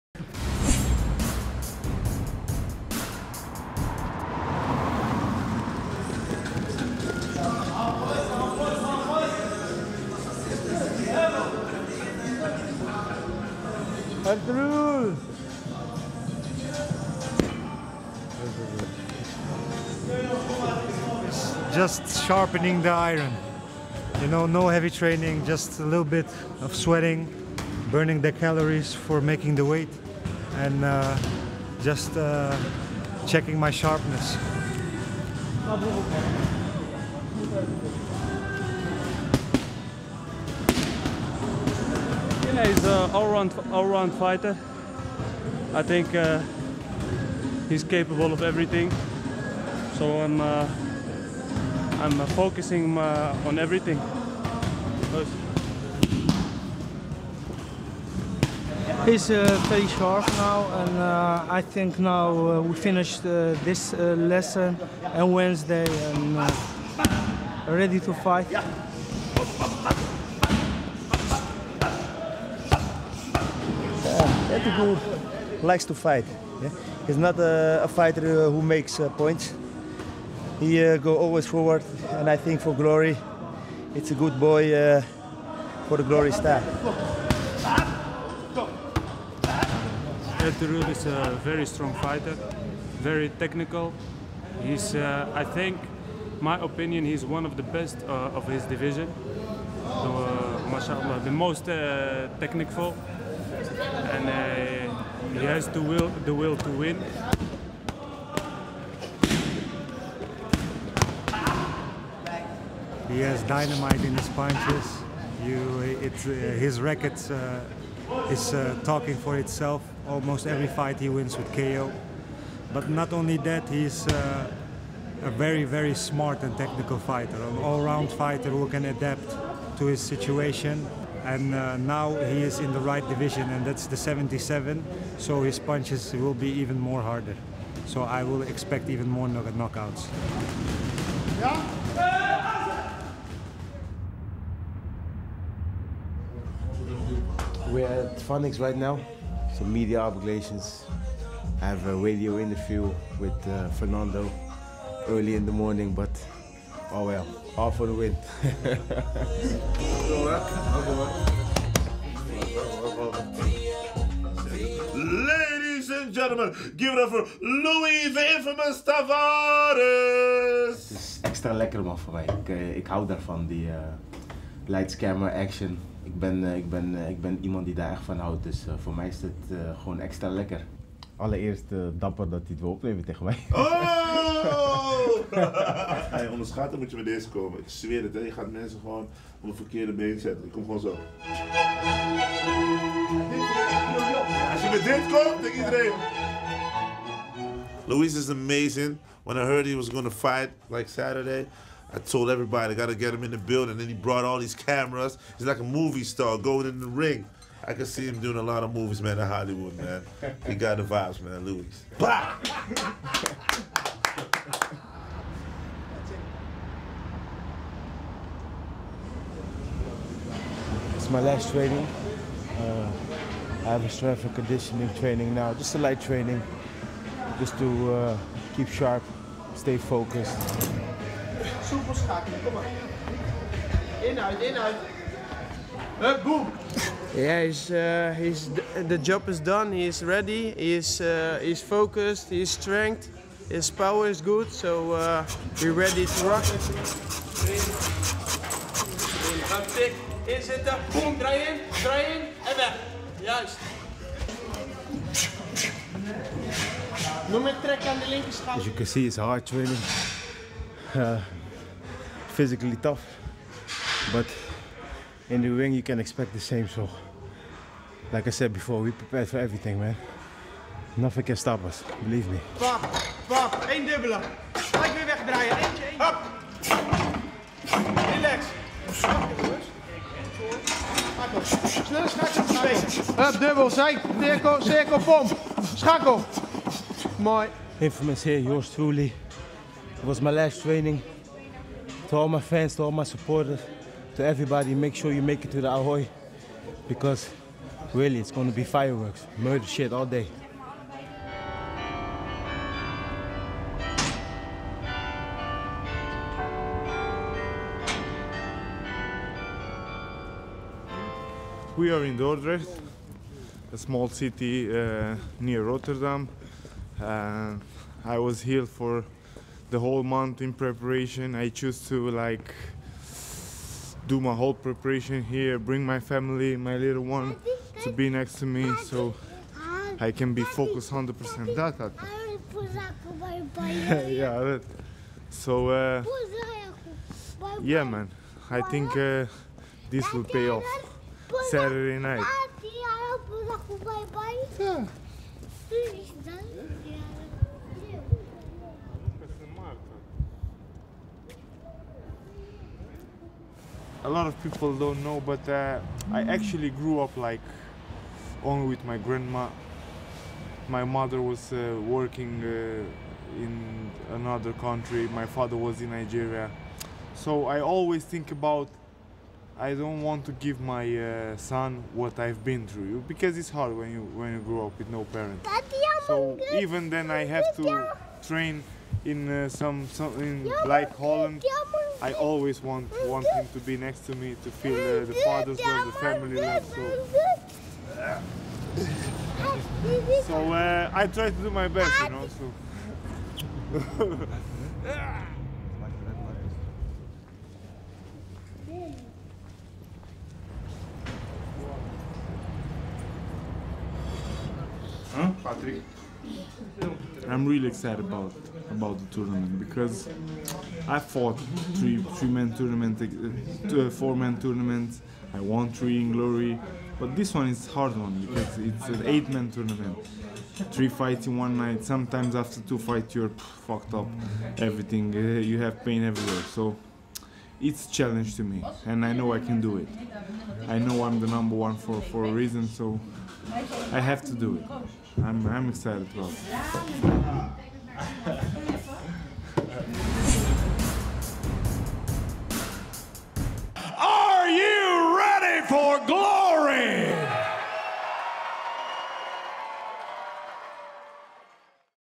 Inside GLORY 62 Rotterdam Fight Week: Part 3
On Part 3 of Inside GLORY 62 Rotterdam Fight Week, teammates Hamicha and Ertuğrul Bayrak get ready for their GLORY debuts, Luis Tavares does media rounds and puts in work during a light training session, and Benjamin Adegbuyi discusses his training camp and the importance of his family.